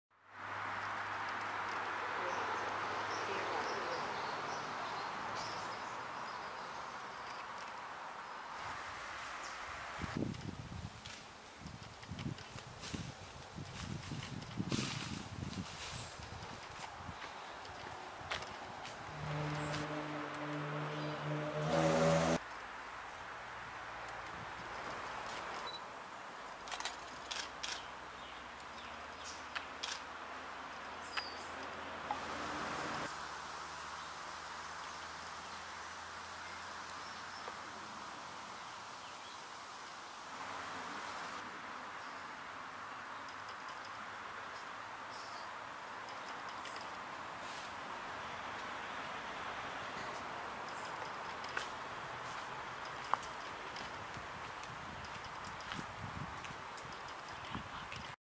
The elusive Australian Platypus
The elusive Australian Platypus having a lovely play in Yungaburra on the Atherton Tablelands. Filmed on our Crater Lakes tour in Far North Queensland